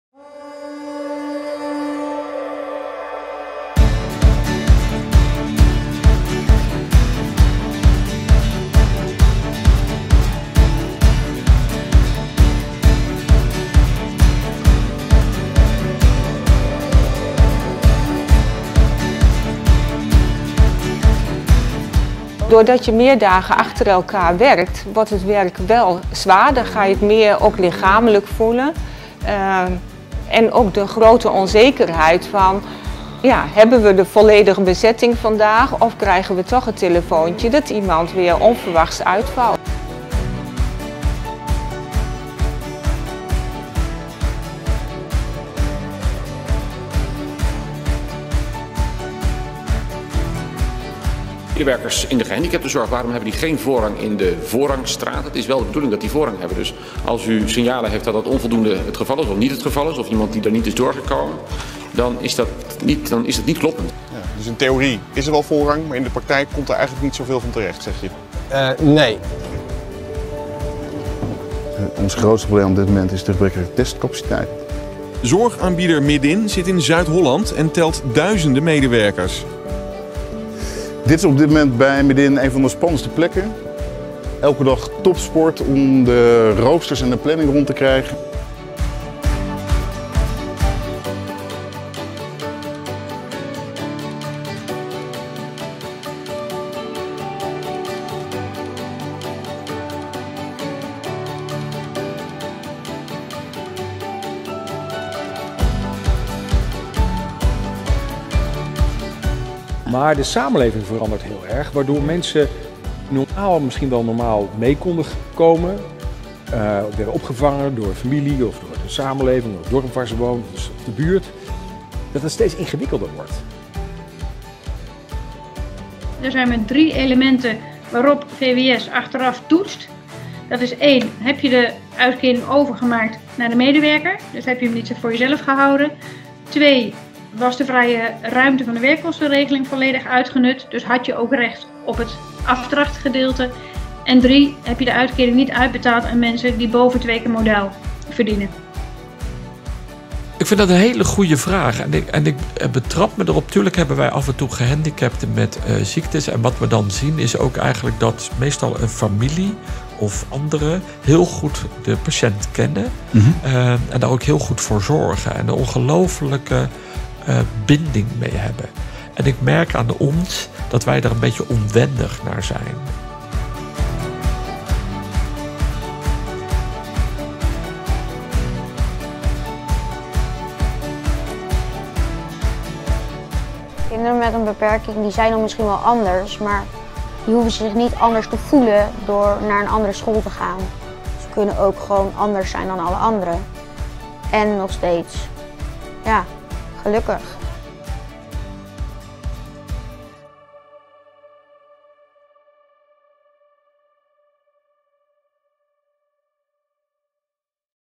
VGN in Beeld juli - november 2020
In de maanden juli t/m november 2020 is de VGN actief geweest op verschillende onderwerpen en thema's. In deze video komen een aantal hoogtepunten in beeld van deze acties en media-momenten. Op lees je meer over de activiteiten van de VGN.